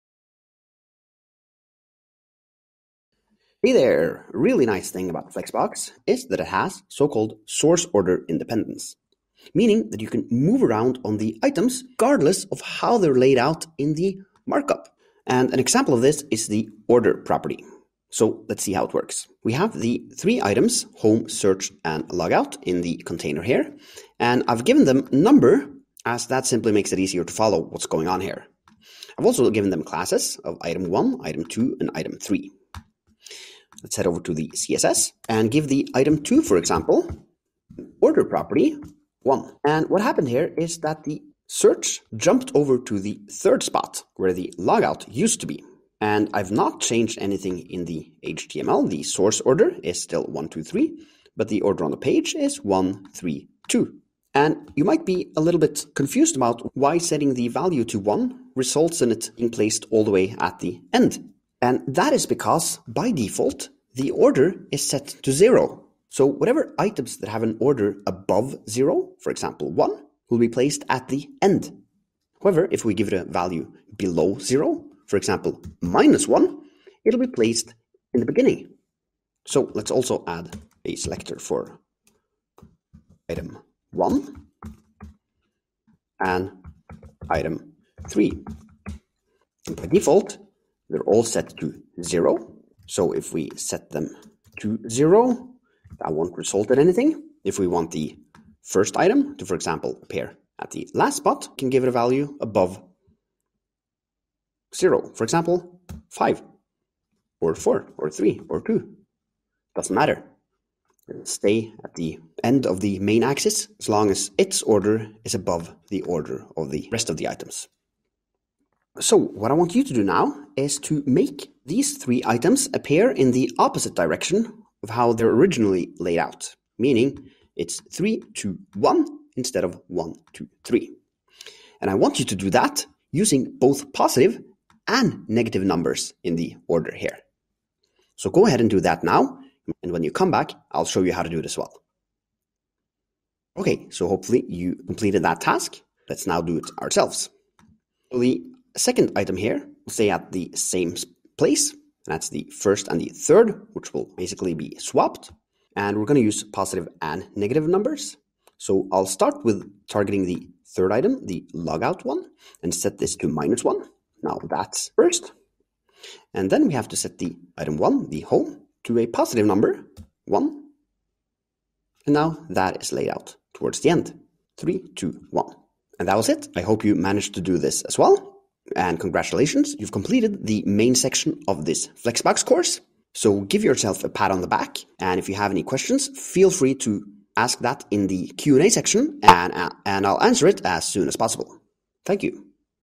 Order | CSS Flexbox tutorial
Ordering allows you to change the order of the items without changing their order in the markup.

Once you've learned CSS Flexbox, you'll wonder how you ever managed to build websites without it. It gives you all the tools you needed in order to properly arrange content inside flexible containers.

Given that all websites need to be flexible and responsive these days, Flexbox is a must-have skill for web developers.